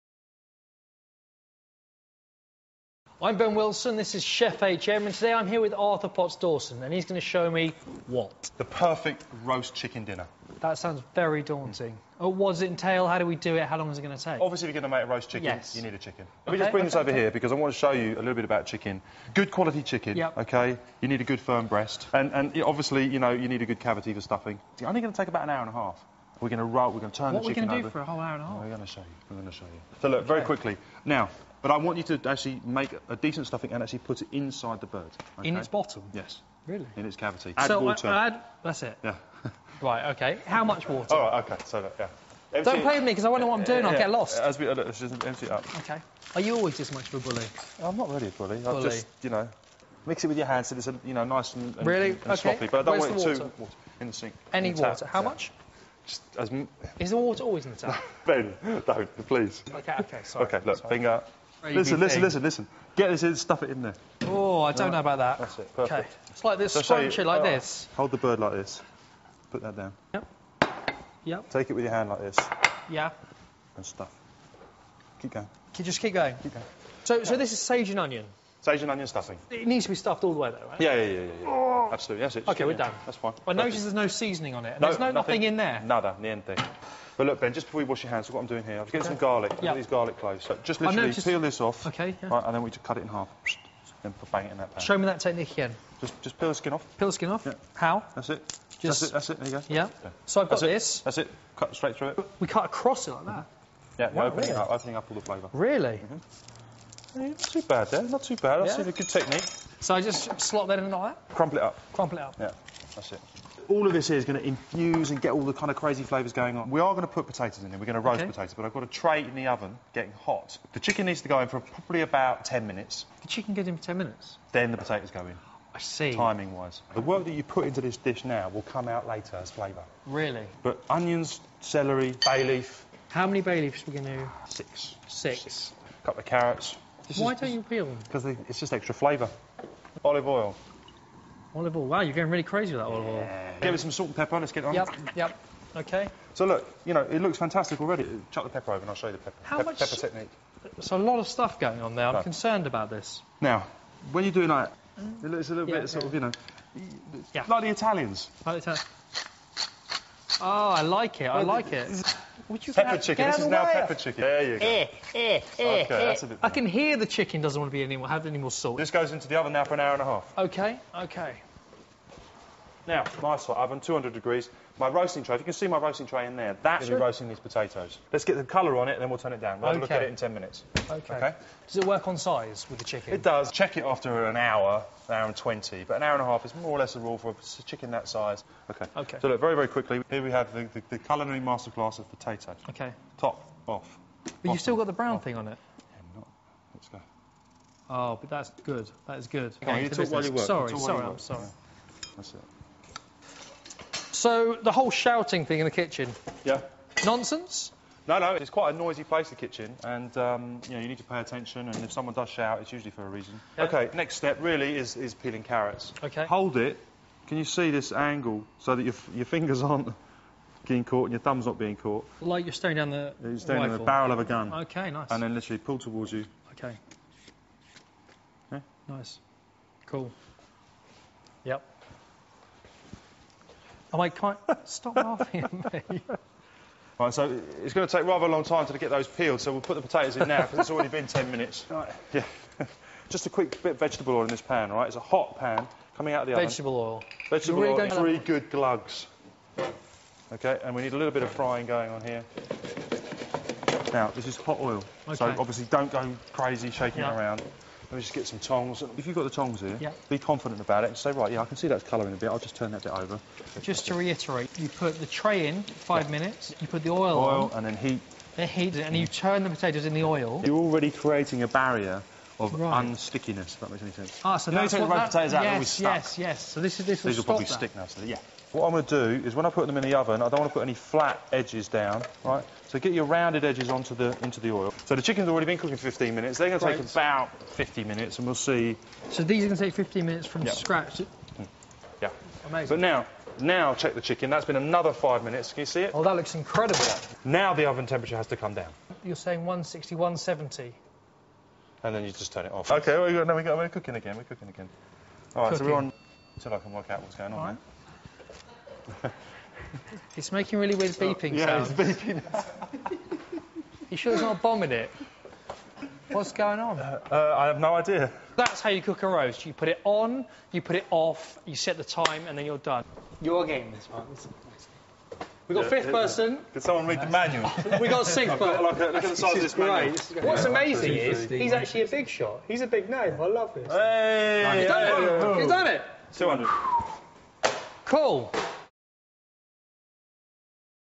Chef FHM - The perfect roast chicken: part one
The latest from Chef FHM: our complete cooking guide for men. FHMs Ben Wilson and Londons award-winning chef Arthur Potts-Dawson take you through how to create the perfect roast from start to finish. Sunday win.